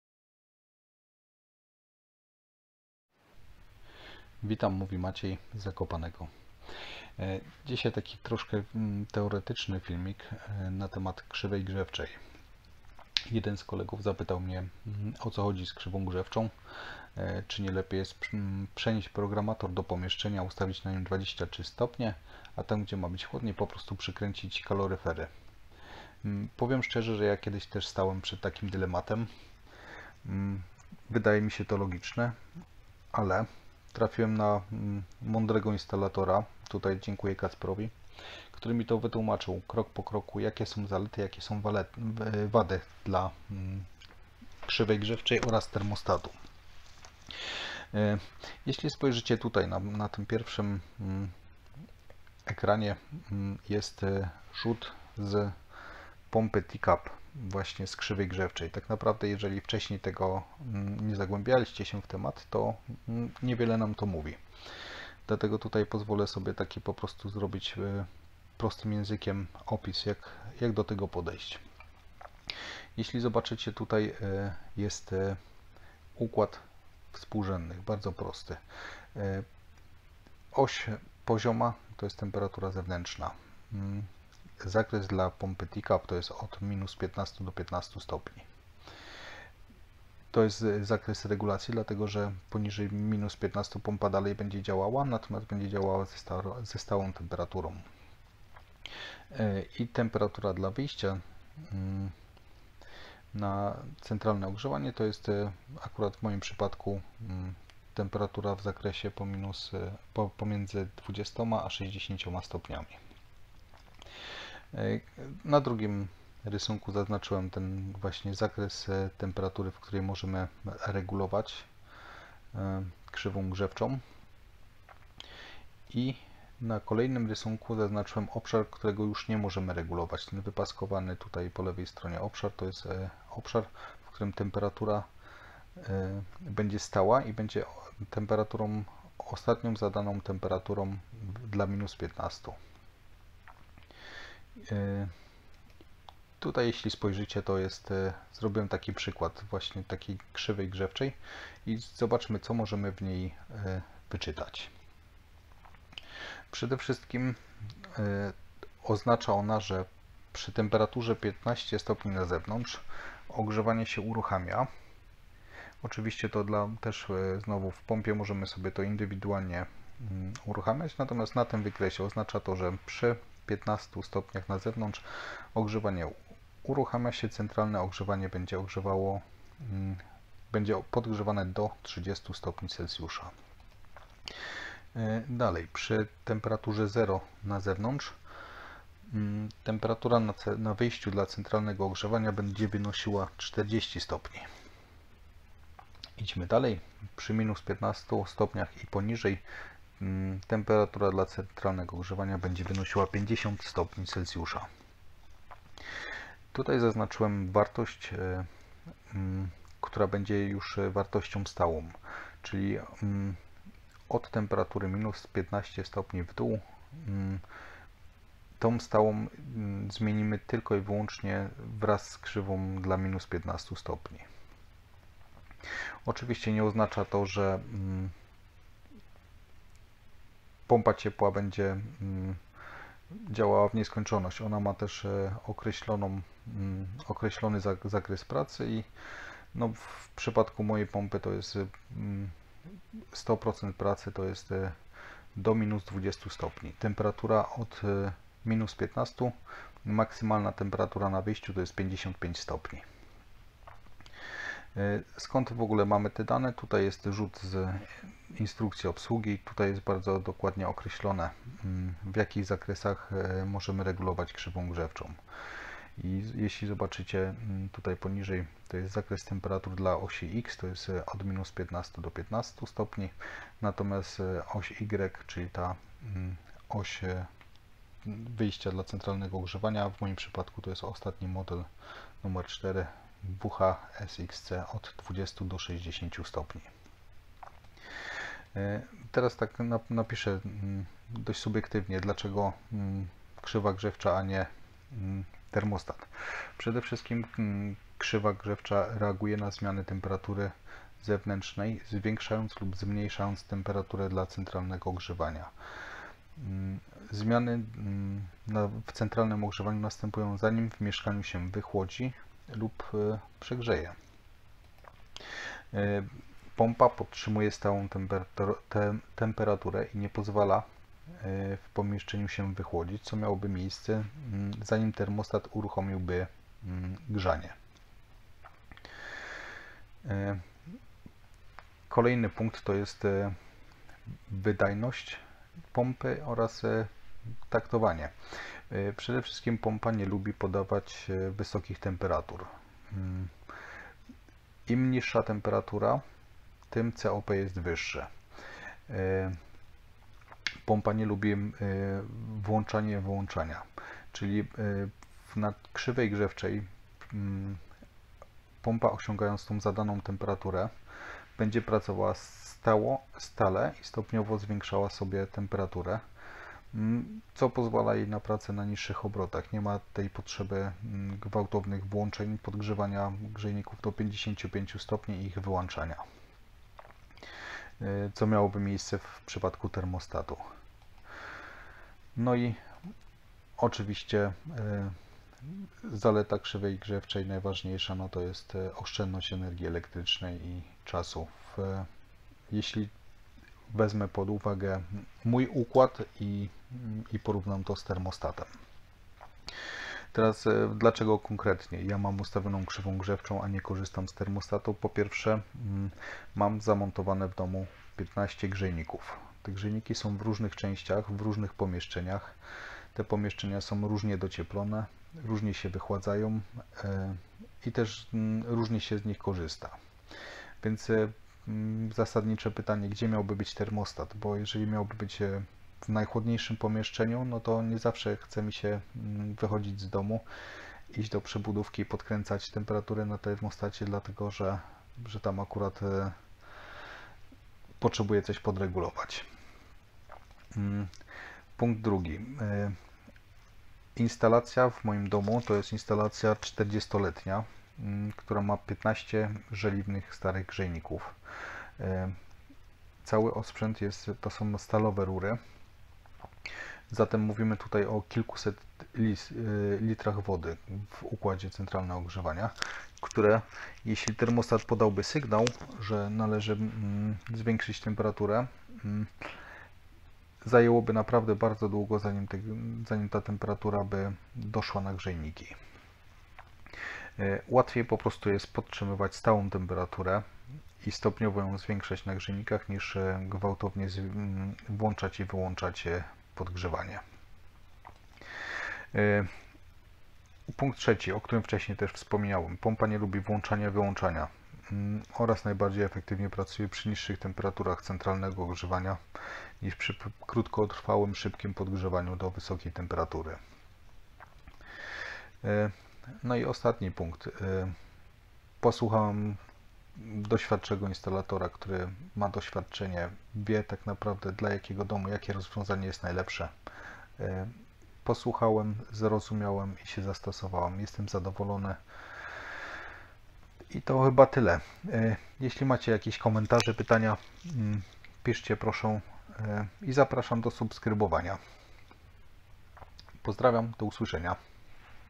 Co to jest krzywa grzewcza, krzywa kompensacji, dlaczego jest dla mnie dobrym rozwiązaniem.
Prosto tłumaczę krzywą kompensacji, oraz dlaczego ją włączyłem.

Krzywa kompensacji wymaga na początku sporo eksperymentowania, nie ma jednego idealnego zestawu parametrów, który będzie działał dla wszystkich domów, czy mieszkań.
W zależności od budynku - temperatura początkowa, przy uruchamianiu CO może być niższa, lub wyższa, do tego możemy zadecydować, że pompa powinna już uruchamiać się przy np. 17°C na zewnątrz ustawiając start na grzejnikach np. na 30°C.
Wraz ze spadkiem temperatury zewnętrznej, być może konkretny budynek nie potrzebuje przy -15°C podawać na grzejniki 55°C, ale np 47°C wystarczy...

Dlaczego warto eksperymentować? Bo nie tylko liczy się komfortowa temperatura w mieszkaniu (kontrolowana częściowo przez termostaty na grzejnikach), ale też niższa temperatura podawana na CO oznacza bardziej efektywną pracę pompy, wyższe COP, a zatem i niższy pobór energii elektrycznej.